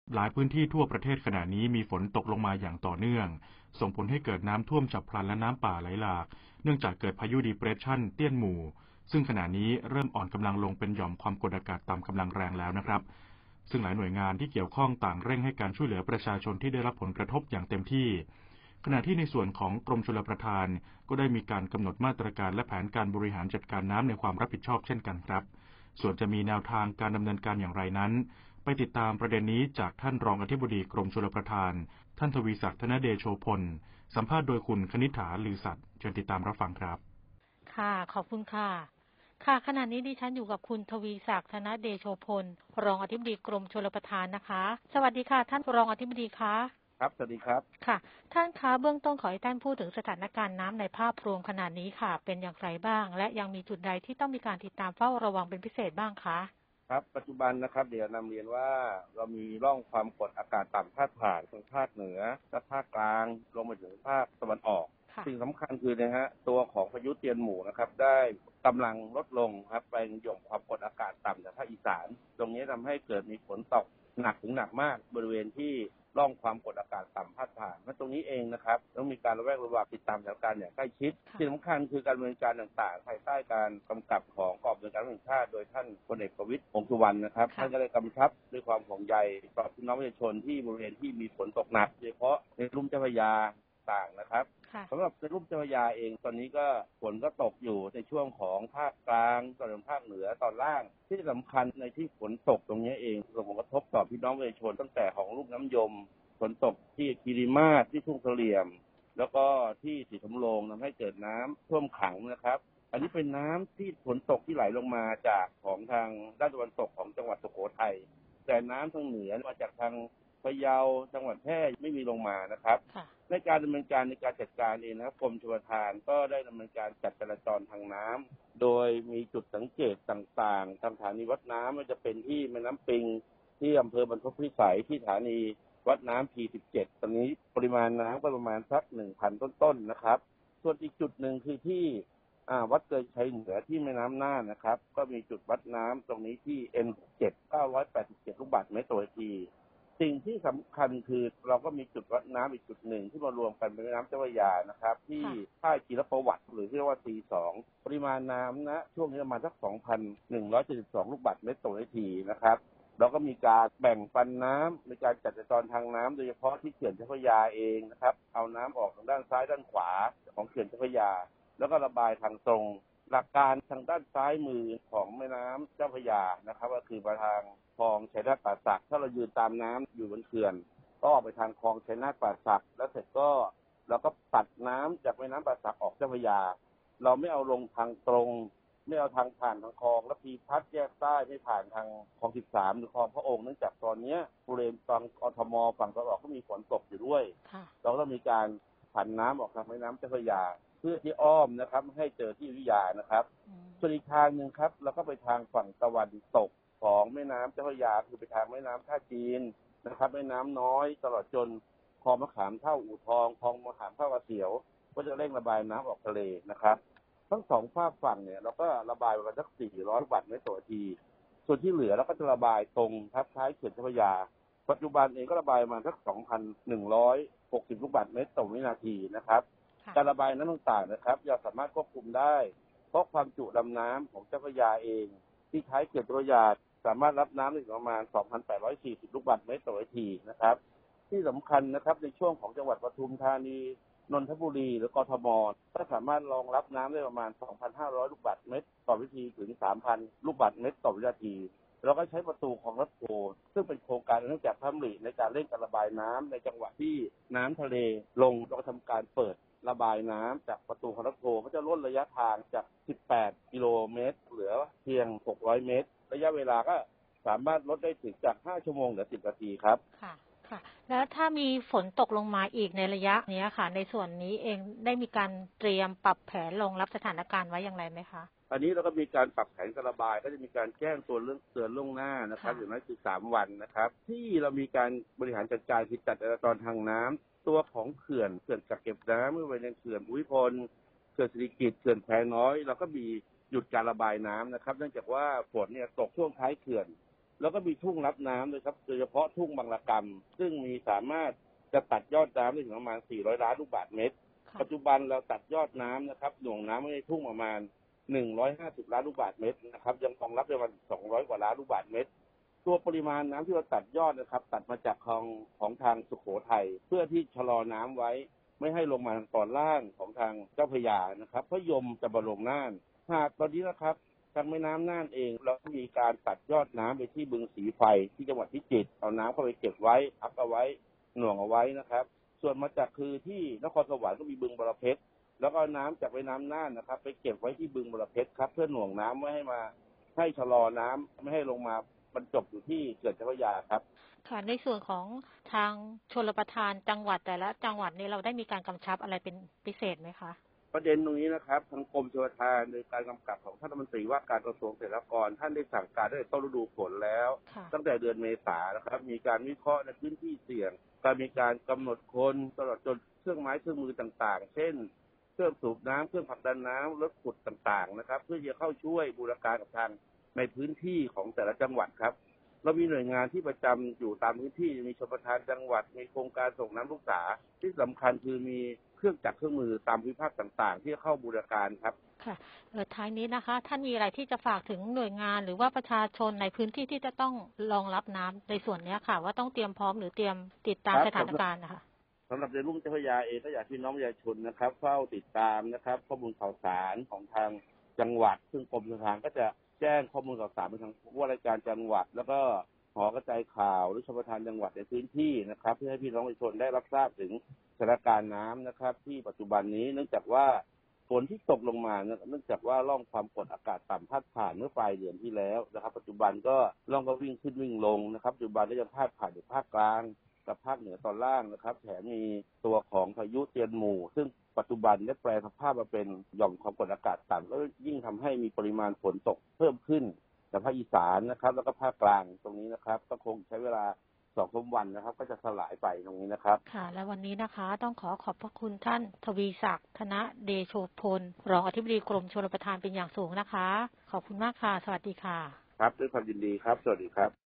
สถานการณ์น้ำในภาพรวมทั่วประเทศ และแผนการระบายน้ำหลังหลายพื้นที่ได้รับผลกระทบจากอิทธิพลพายุเตี้ยนหมู่
นายทวีศักดิ์ ธนเดโชพล รองอธิบดีกรมชลประทาน เผย สถานการณ์น้ำในภาพรวมทั่วประเทศ และแผนการระบายน้ำหลังหลายพื้นที่ได้รับผลกระทบจากอิทธิพลพายุโซนร้อน "เตี้ยนหมู่" ออกอากาศภาค 07.30 น วันที่ 26 ก.ย.64 โดย สวท.กทม.